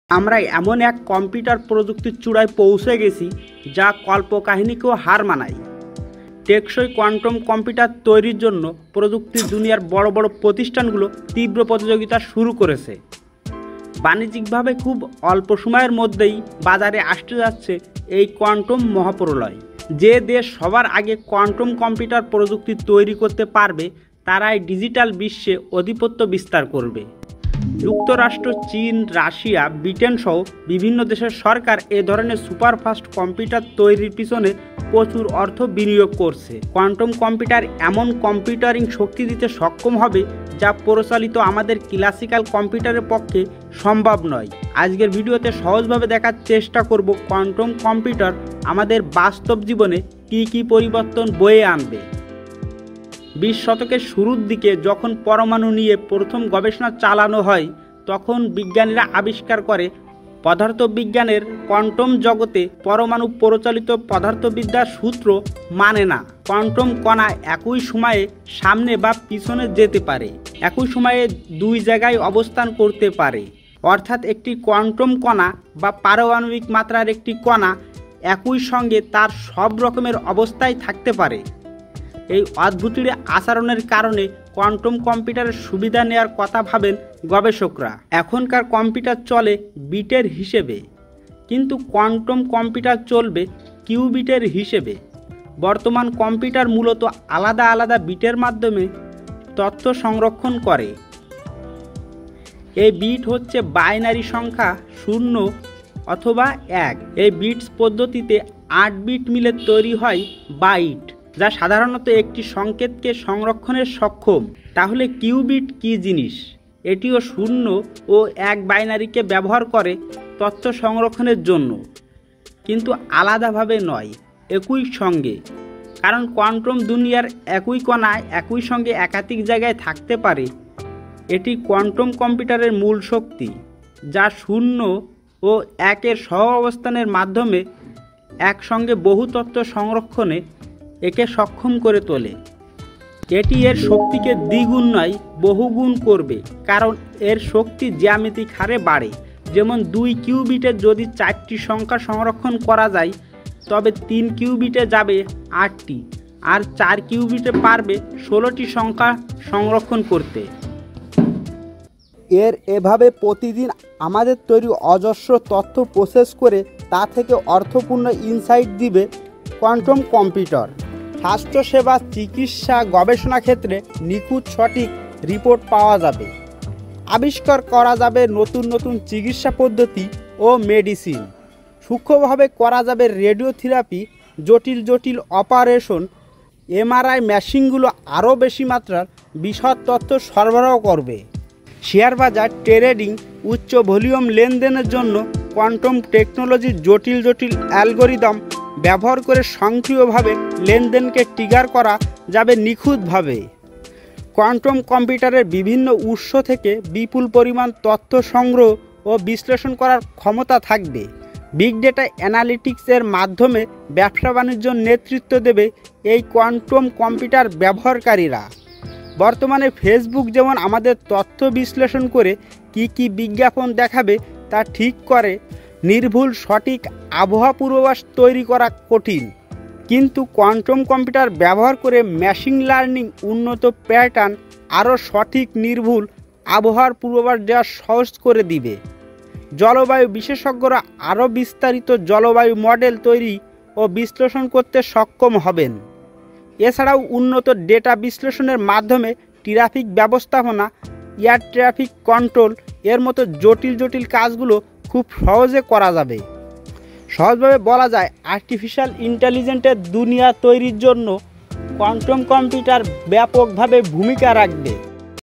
কোয়ান্টাম কম্পিউটার বাস্তব জীবনে কি কি পরিবর্তন বয়ে আনবে।
তারাই ডিজিটাল বিশ্বে আধিপত্য বিস্তার করবে যে দেশ সবার আগে কোয়ান্টাম কম্পিউটার তৈরি করতে পারবে .